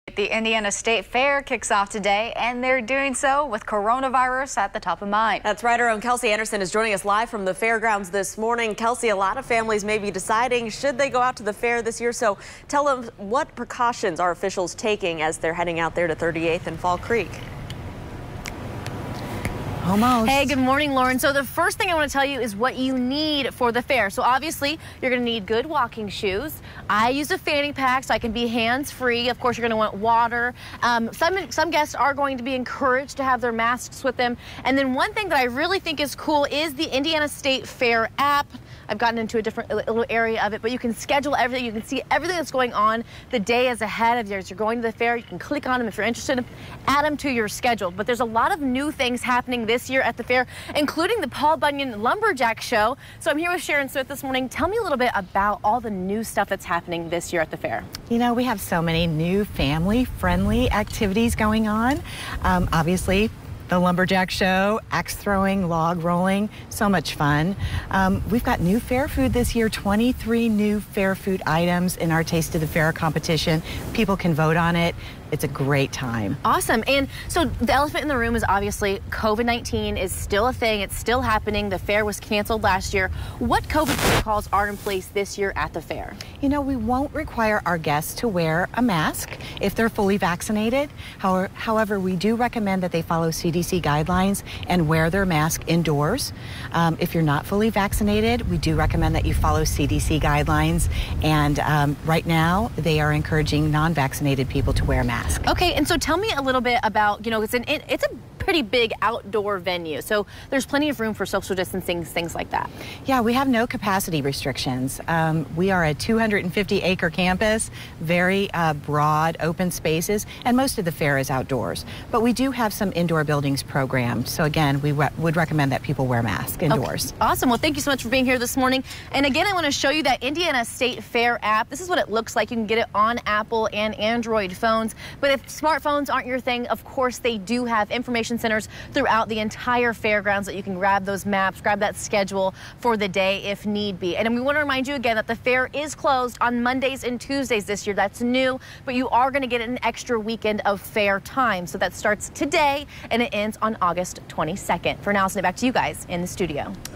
Indiana State Fair kicks off with COVID-19 precautions, new features
There are several new activities, events and features at the Indiana State Fair this year.